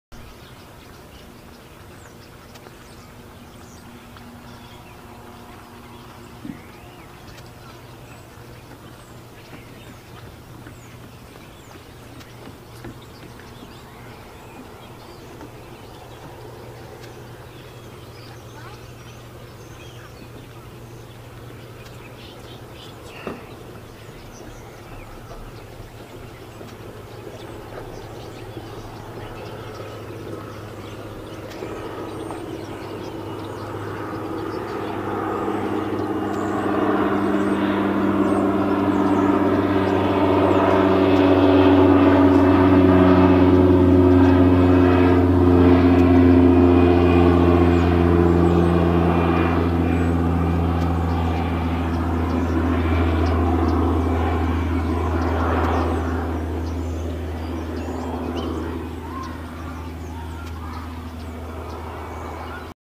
Dragonfly laying eggs
At Terranova pond Sunday 27 August 2017